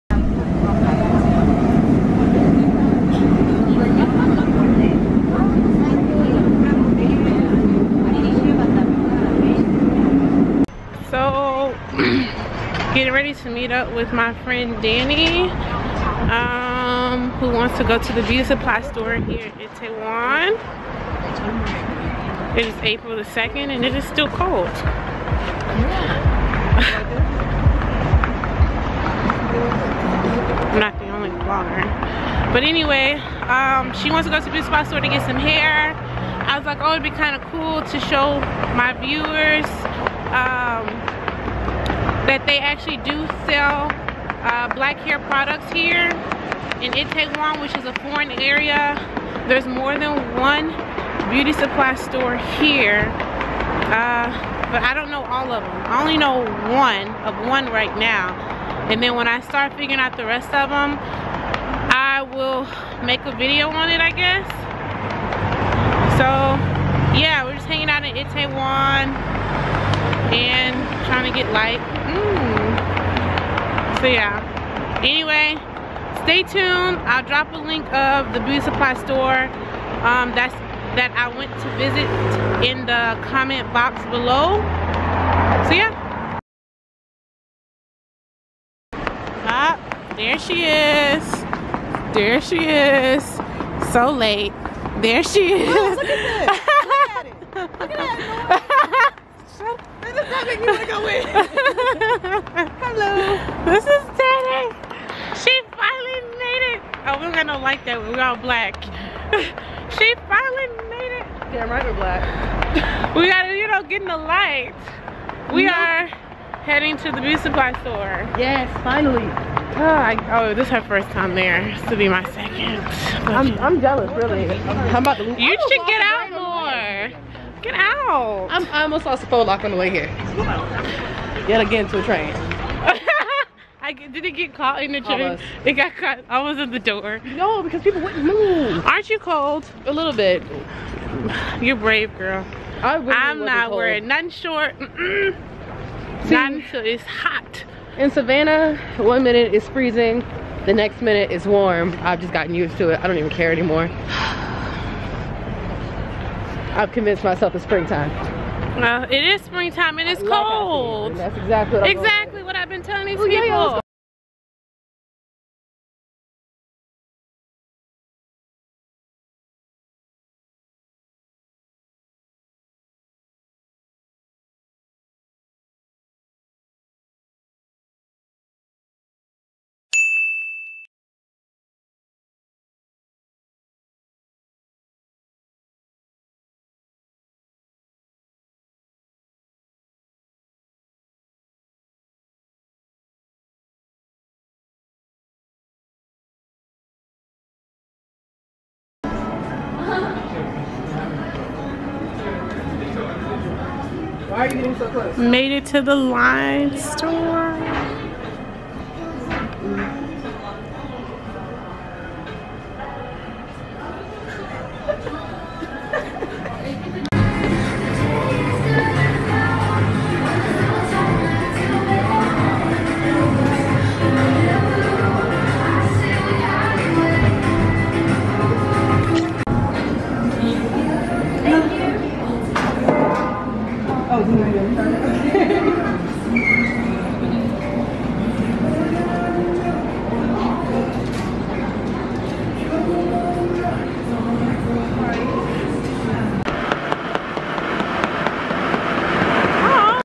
VLOG 6 Hanging Out In Itaewon
After work, I met up with a friend in Itaewon to go to a black beauty supply store.

 Beauty Supply Store located at Exit 4 Itaewon Station